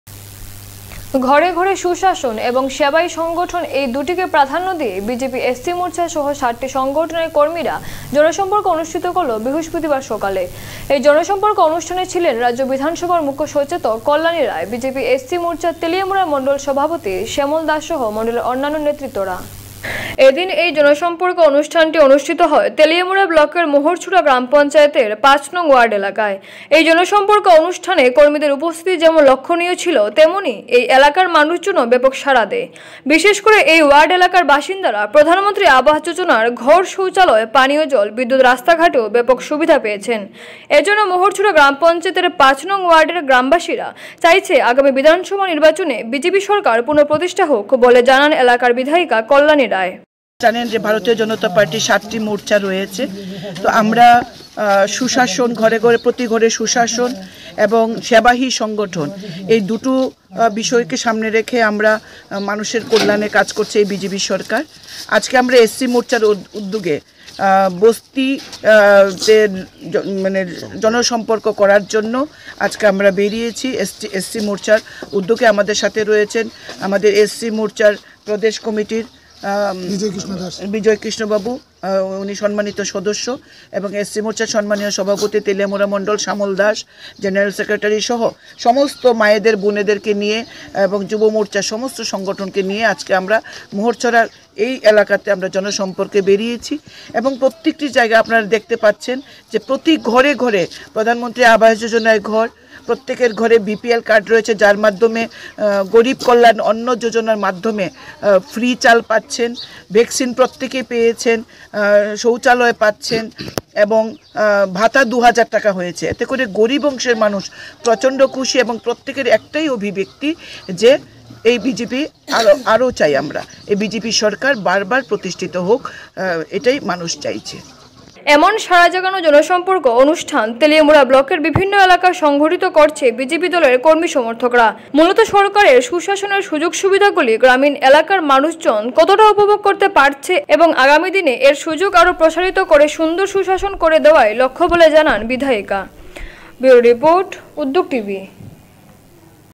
বিজেপি এস.সি মোর্চা সহ সাতটি সংগঠনের কর্মীরা জনসম্পর্ক অনুষ্ঠিত করল বৃহস্পতিবার সকালে
ঘরে ঘরে সুশাসন এবং সেবাই সংগঠন এই দুইটিকে প্রাধান্য দিয়ে বিজেপি এস.সি মোর্চা সহ সাতটি সংগঠনের কর্মীরা জনসম্পর্ক অনুষ্ঠিত করল